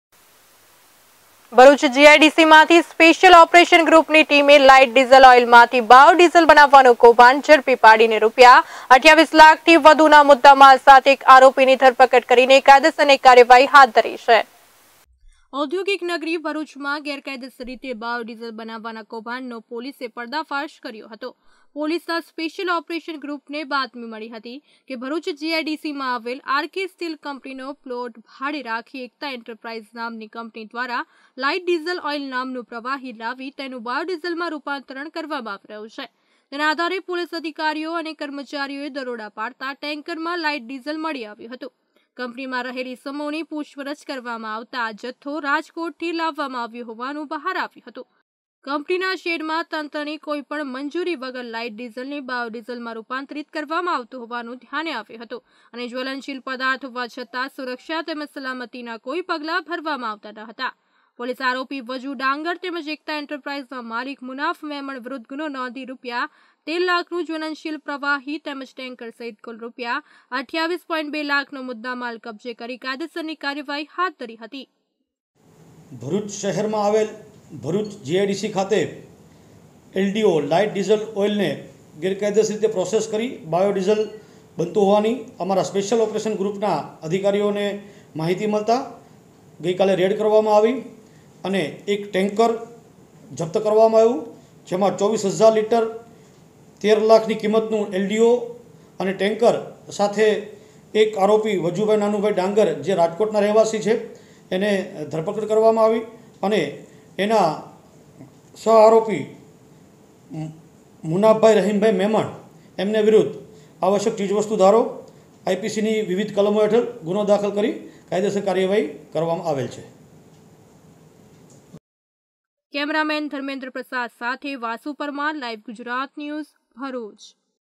ભરૂચ: લાઇટ ડીઝલ ઓઇલમાંથી બાયોડીઝલ બનાવવાનું કૌભાંડ ઝડપાયું
ભરૂચ જીઆઈડીસીમાંથી સ્પેશ્યલ ઓપરેશન ગ્રૂપની ટીમે લાઇટ ડીઝલ ઓઇલમાંથી બાયો ડીઝલ બનાવવાનું કૌભાંડ ઝડપી પાડી રૂપિયા 28 લાખથી વધુના મુદામાલ સાથે એક આરોપીની ધરપકડ કરી કાયદેસરની કાર્યવાહી હાથ ધરી છે.